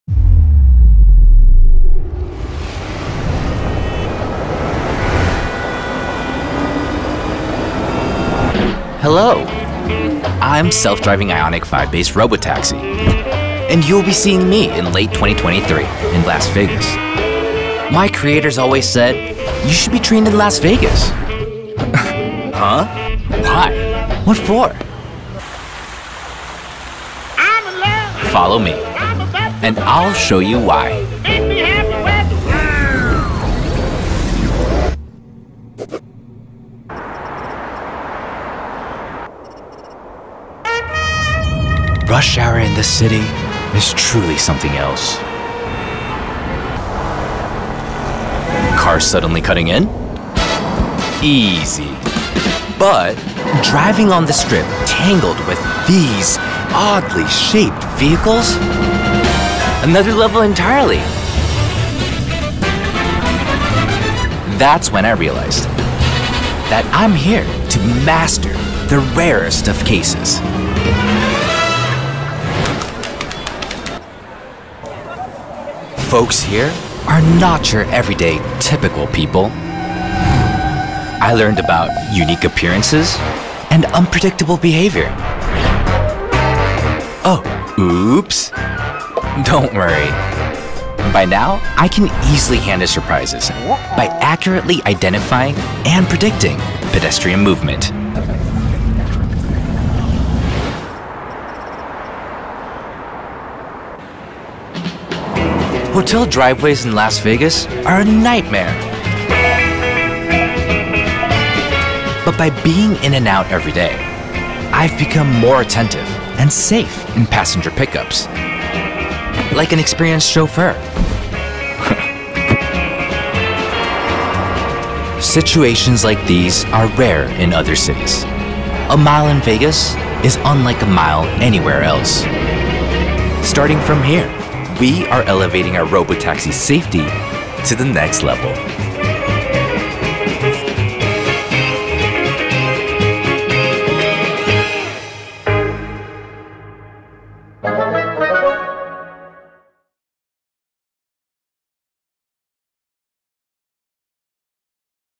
Robotaxi Ioniq 5 - Las Vegas⁣
¿Sabes por qué nuestro Ioniq 5 debutó en Las Vegas como el Robotaxi?⁣

La reconocida ciudad del entretenimiento mundial fue la elegida para tener el debut de nuestro Ioniq 5 como Robotaxi de Hyundai. Una de las ciudades más movidas del mundo, que además tiene la particularidad de que sus millas no son las mismas millas que en otro lugar del mundo. Es por eso por lo que nuestros expertos decidieron que entre más conduzcamos en caminos difíciles, más seguro será el viaje del Ioniq 5.⁣
Vive cada momento del proyecto robotaxi en Las Vegas aquí.⁣